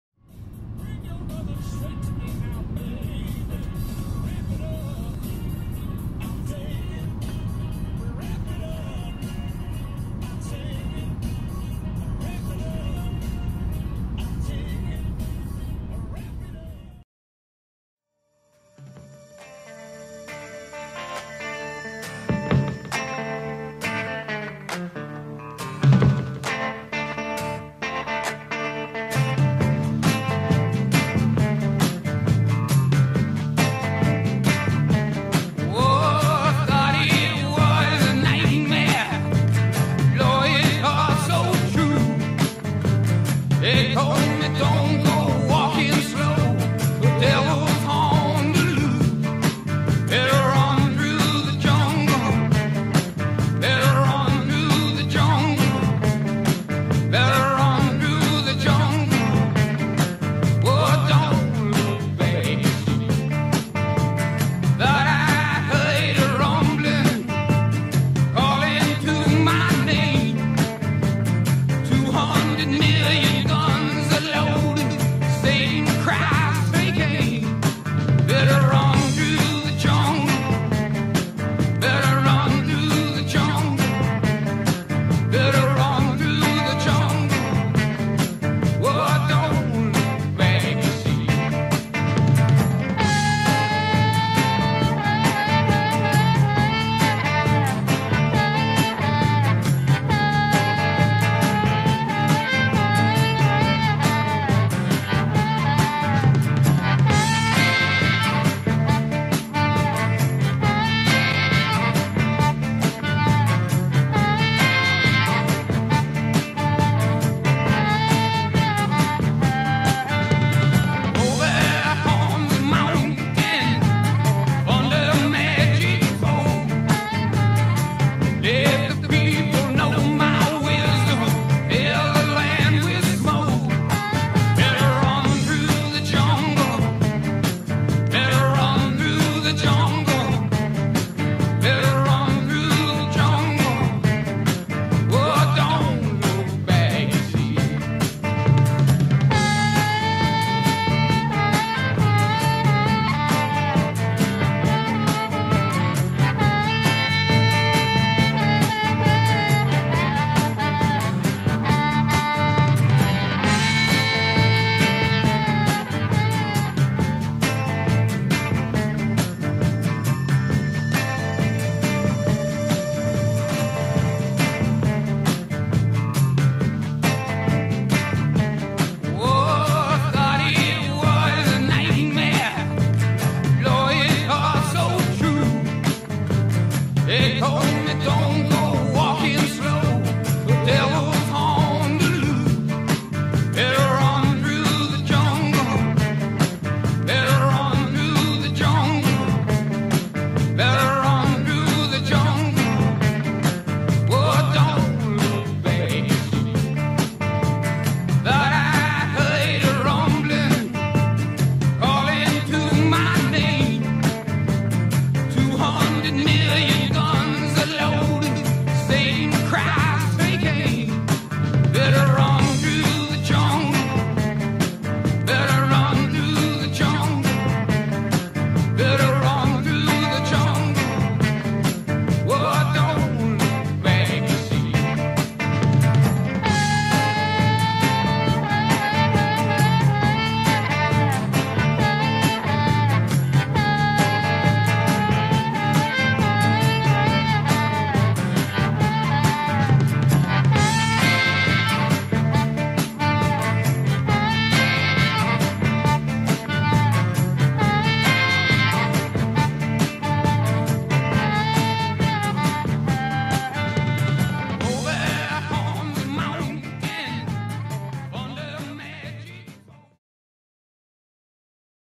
Canada 2017 (Othello tunnels) #2
První nedělní výlet do Othello tunnels, kousek od města Hope.

First sunday trip to the Othello tunnels near the city Hope.